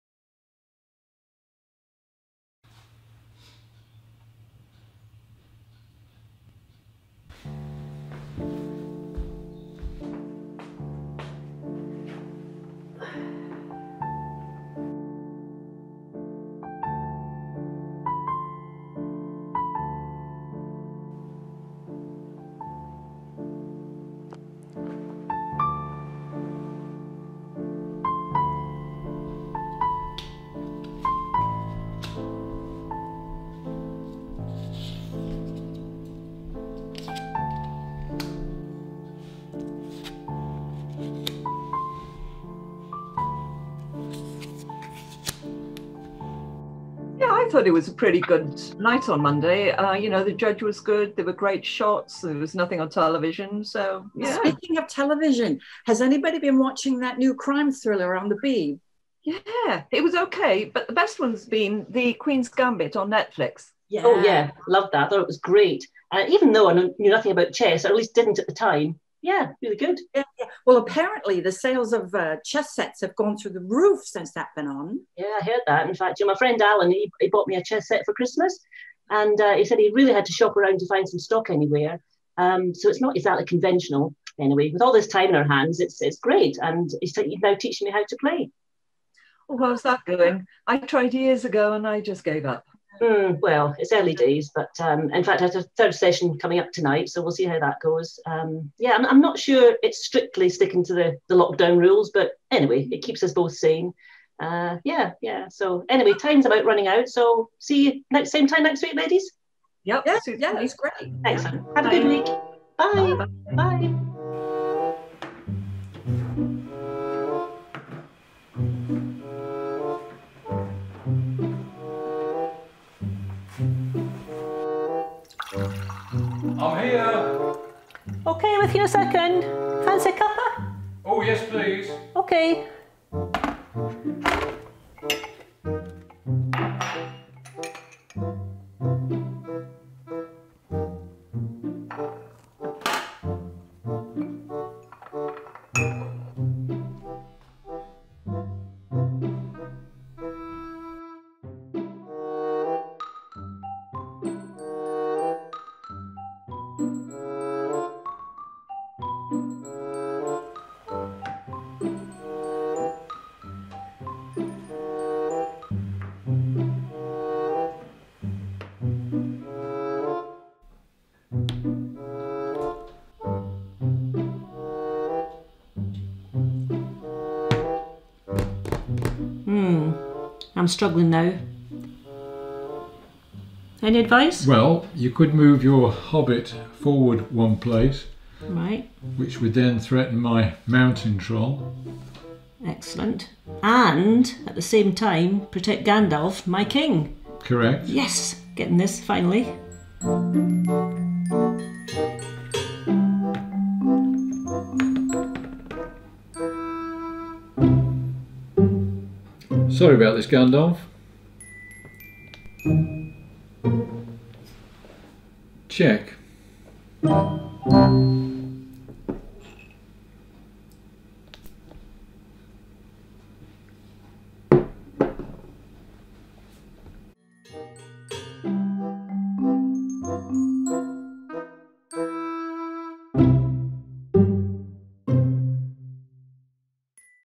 Tea for Two
Winner of 3 Stars at BIAFF 2021. Stuck at home during Lockdown 3, I wrote this script to enter into my local film club 4 min competition. Home alone meant no actors could join me. so how could I get round that?